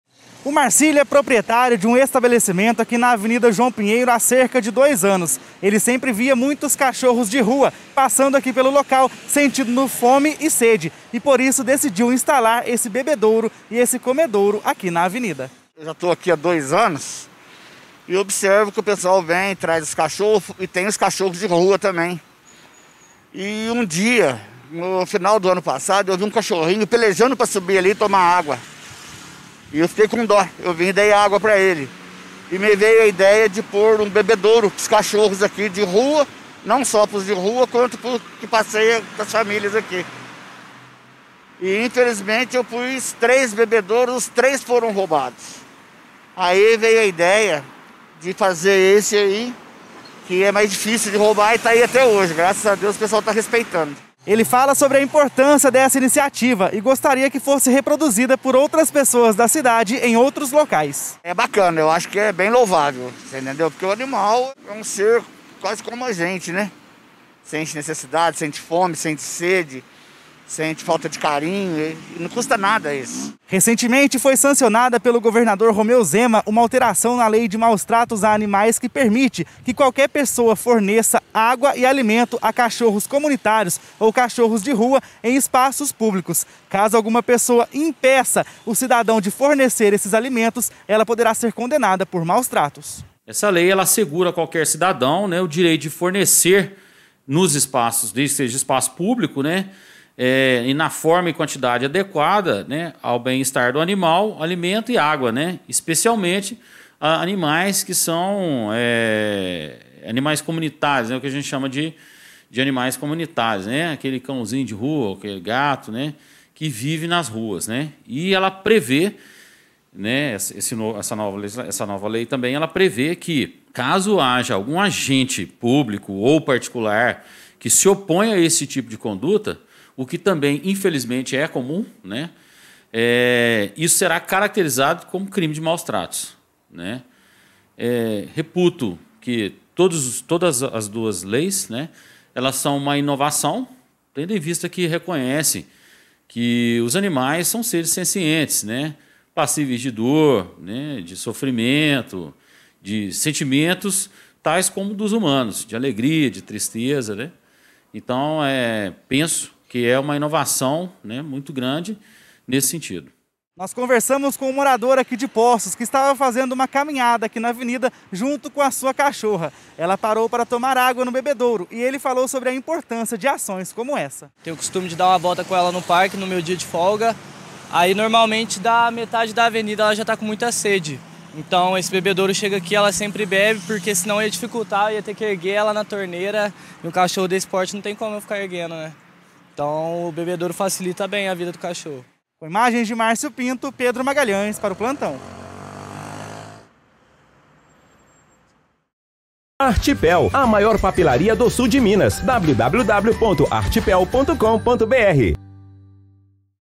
ÁGUA E COMIDA PARA ANIMAIS DE RUA, EM POÇOS DE CALDAS
Em alguns locais da cidade, os moradores vêm dispondo potes com ração e água, aos animais que vivem na rua.